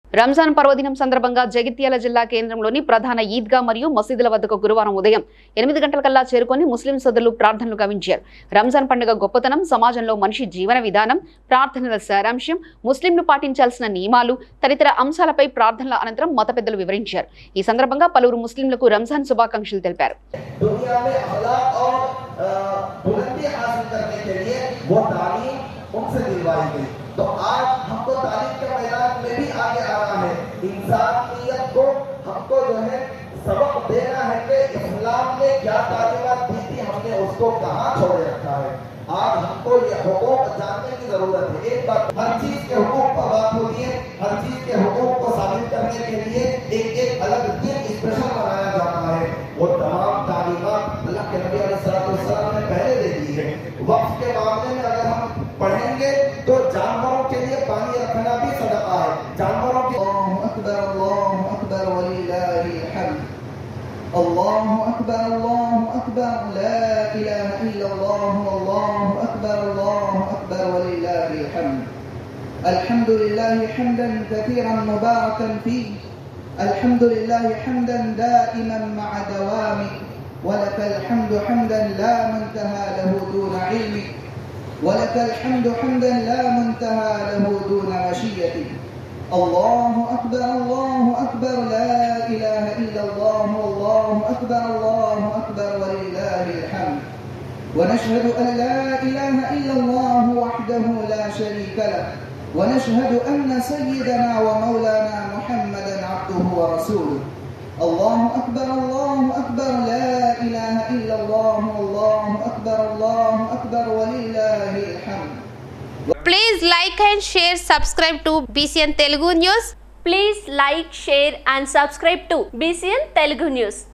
రంజాన్ పర్వదినం సందర్భంగా ఈద్గా & మసీదుల వద్ద ప్రార్థనలు గావించిన : ముస్లిం సోదరులు
రంజాన్ పర్వదినం సందర్భంగా జగిత్యాల జిల్లా కేంద్రంలోని  ప్రధాన ఈద్గా మరియు మసీదుల వద్దకు గురువారం ఉదయం 8 గంటలకల్లా చేరుకుని ముస్లిం సోదరులు ప్రార్థనలు గావించారు. రంజాన్ పండుగ గొప్పతనం, సమాజంలో మనిషి జీవనవిధానం, ప్రార్థనల సారాంశం, ముస్లింలు పాటించాల్సిన నియమాలు తదితర అంశాలపై ప్రార్థనల అనంతరం మతపెద్దలు వివరించారు. ఈ సందర్భంగా పలువురు  ముస్లింలకు రంజాన్ శుభాకాంక్షలు తెలిపారు.